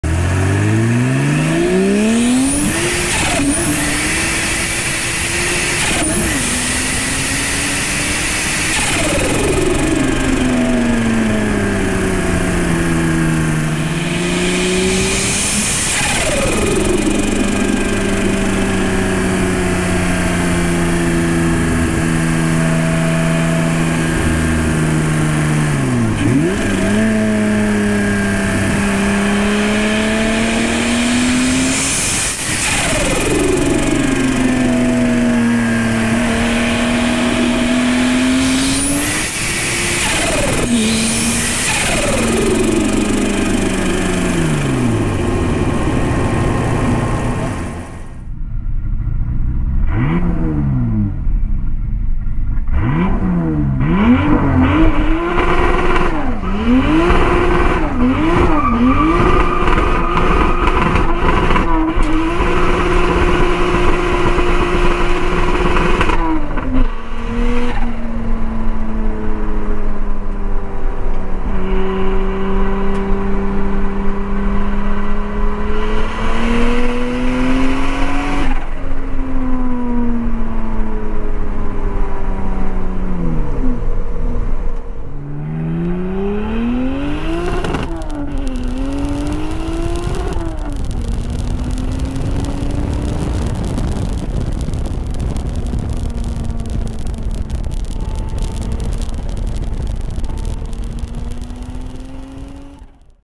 GoPro hero 7 external mic Turbo E30 test
Decided to try out the new gopro hero 7 black, really happy with picture quality and how smooth it is, but sense of speed is a little lacking because it doesnt feature the superwide mode of the older go pros. Using a hero 6 mic adapter and generic microphone.